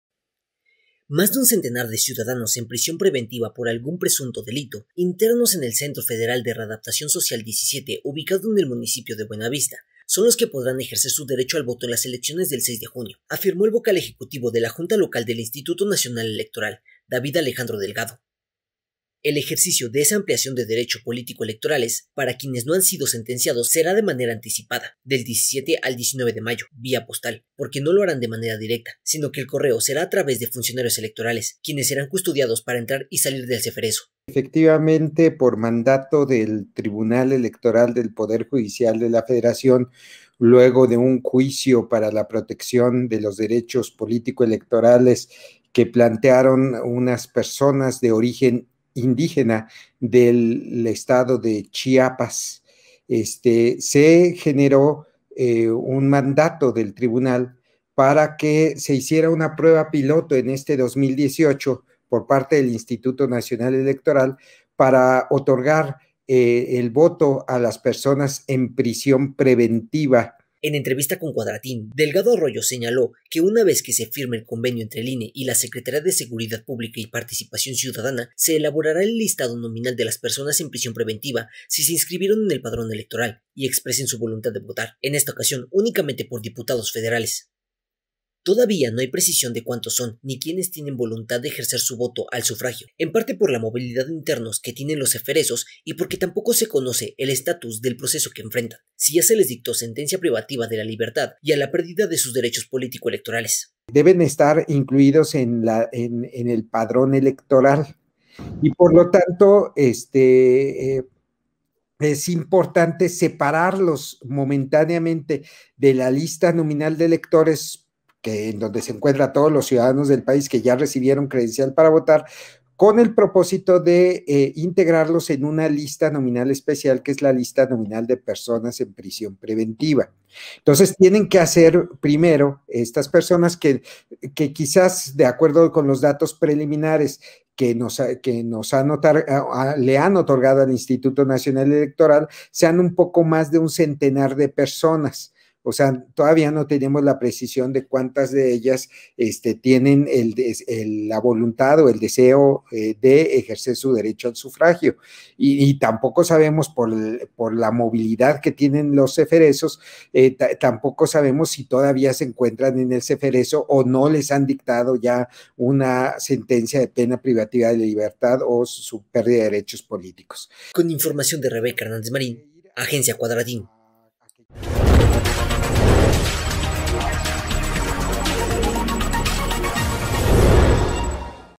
Votarán más de 100 reclusos del Cereso de Buenavista el 6 de junio
MORELIA, Mich., 7 de marzo de 2021.- Más de un centenar de ciudadanos en prisión preventiva por algún presunto delito, internos en el Centro Federal de Readaptación Social 17 ubicado en el municipio de Buenavista, son los que podrá ejercer su derecho al voto en las elecciones del seis de junio, afirmó el vocal ejecutivo de la Junta Local del Instituto Nacional Electoral (INE), David Alejandro Delgado.